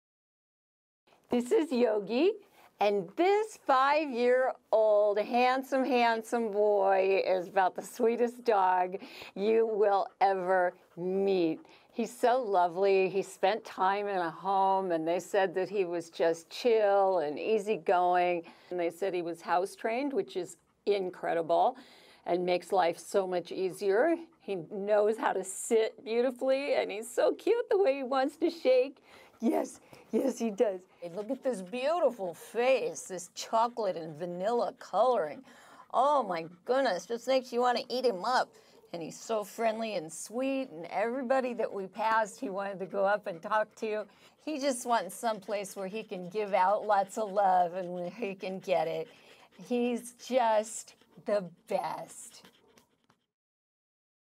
Adopt Yogi at NKLA
Meet 5 yr old Yogi.

For more information please:
visit: 1845 Pontius Ave. Los Angeles, CA, 90025